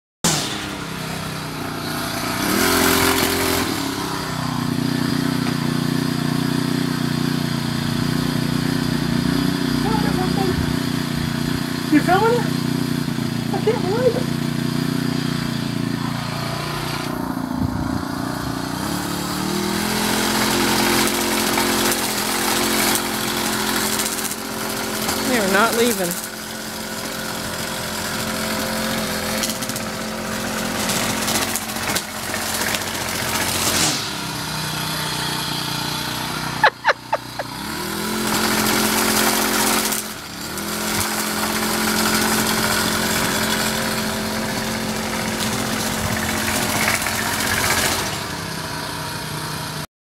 Pine Siskins will NOT move for yard work
After spending time taking pictures and actually touching these birds, they refuse to leave the feeder even when we can't wait any longer to run the weed eater.  They stayed there for the large riding mower after this video was over.